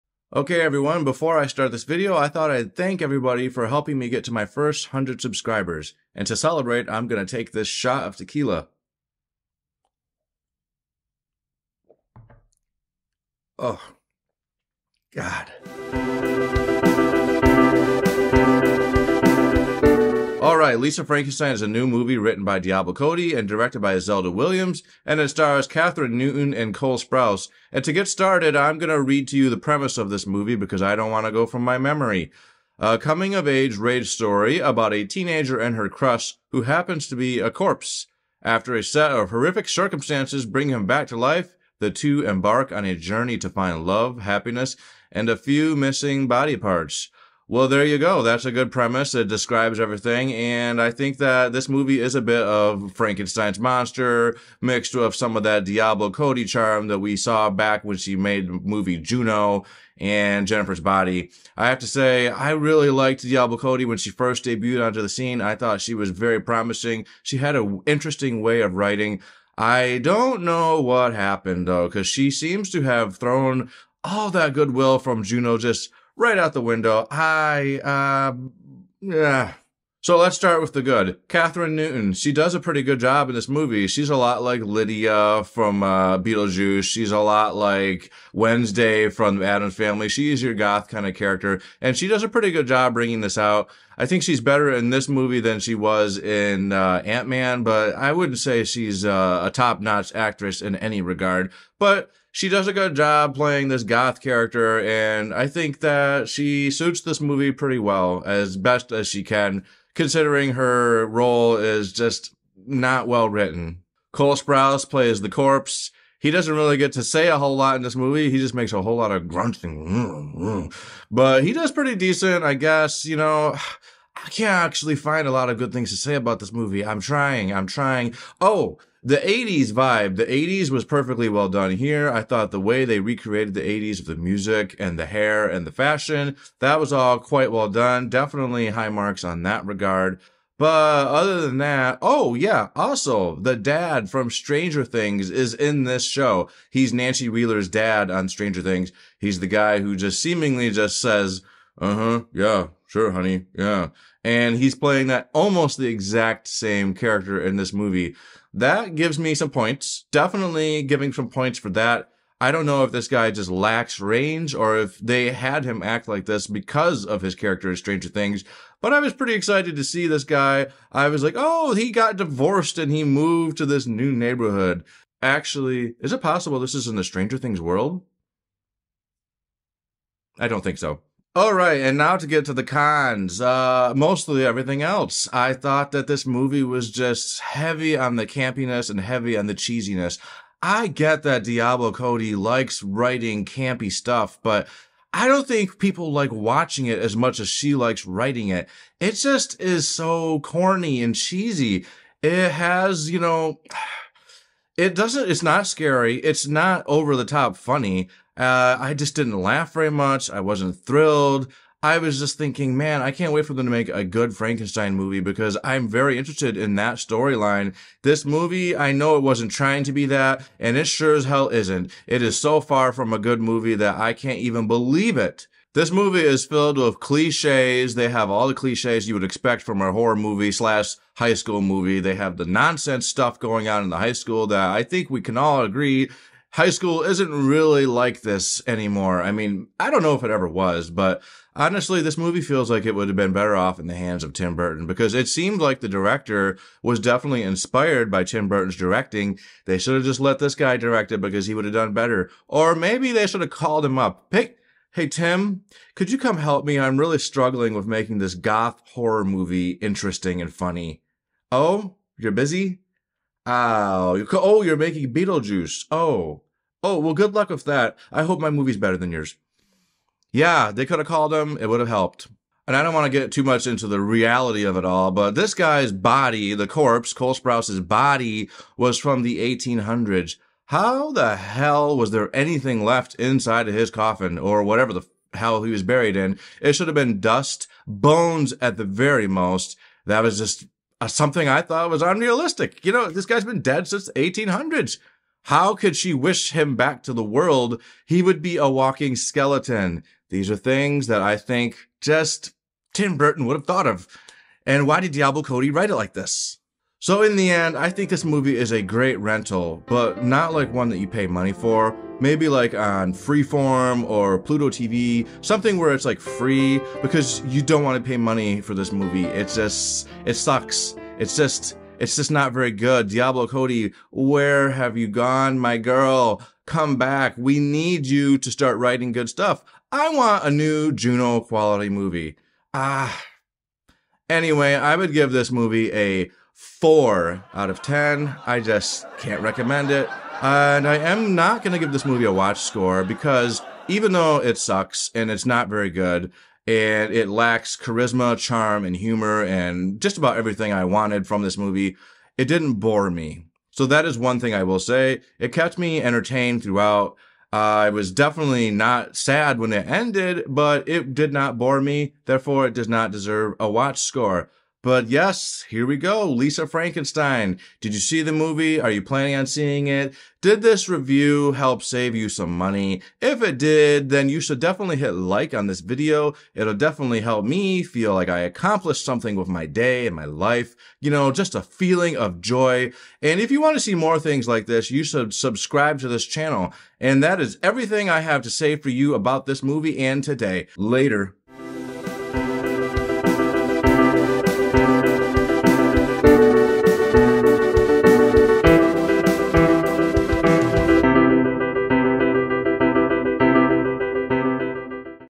A Testament to shit: Lisa Frankenstein Movie Review
Lisa Frankenstein is a new movie written by Diablo Cody (Juno, Jennifer's Body) and stars Kathryn Newton(Antman) and Cold Sprouse and as my title suggests, this movie is shit. This is not the new release you were hoping for but then again it is February so we've got slim pickens. Let's discuss what went wrong!